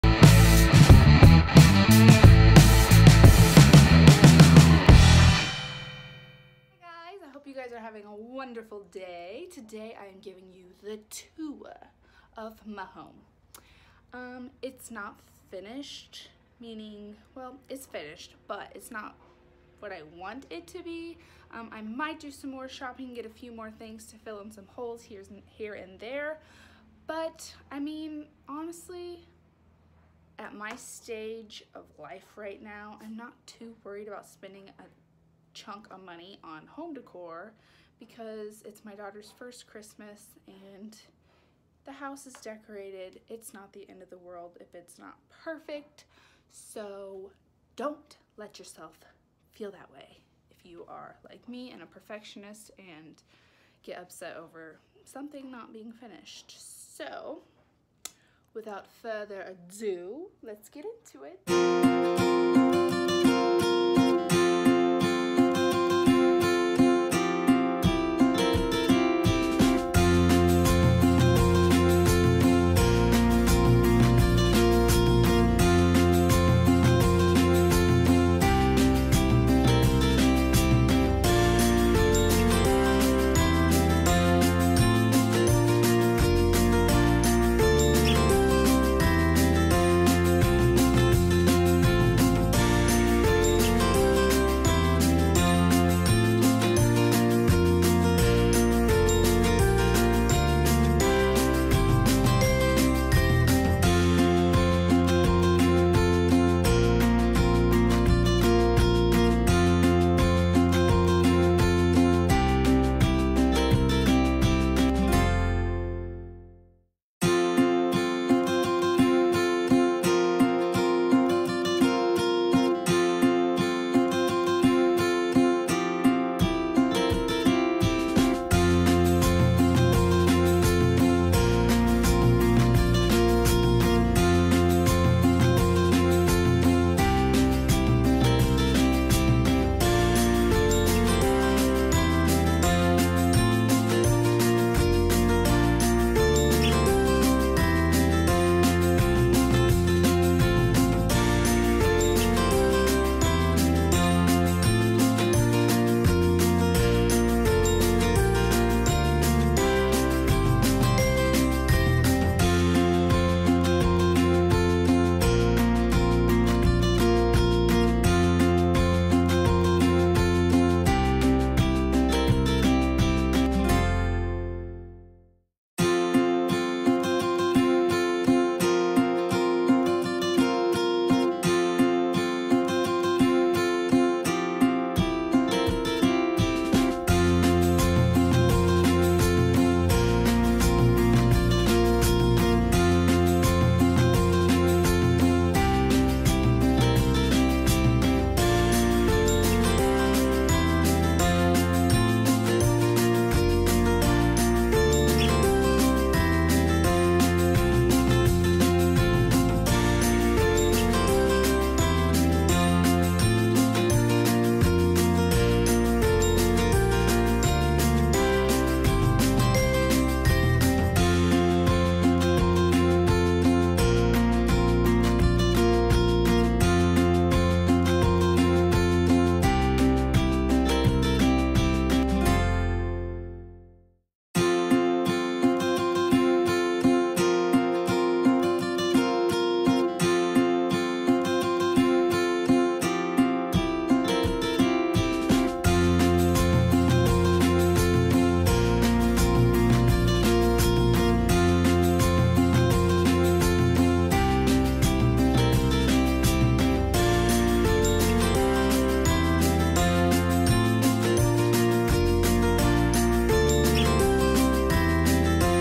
CHRISTMAS HOUSE TOUR | vlogmas day 1
Hey sweets! I know it’s simple but it’s our home! One day I will decorate from top to bottom!